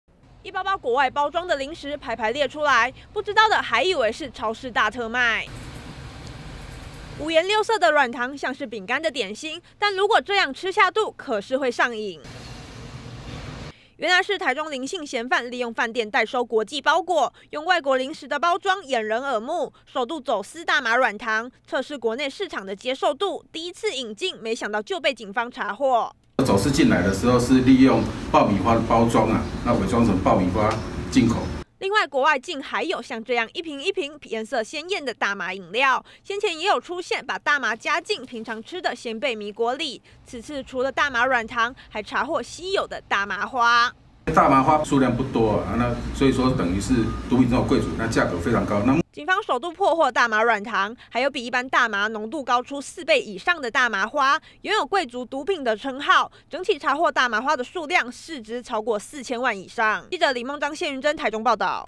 20181016中天新聞 台灣首度破獲大麻軟糖、大麻花 市值逾4千萬
大麻竟然可以變成軟糖？先前就出現過大麻餅乾跟飲料，這次台中一名林姓嫌犯，利用飯店代收國際包裹，首度走私大麻軟糖，想測試國內的接受度，同時破獲的還有號稱貴族毒品的大麻花，它的濃度比一般大麻高出4倍以上，總市值達到了4000萬以上。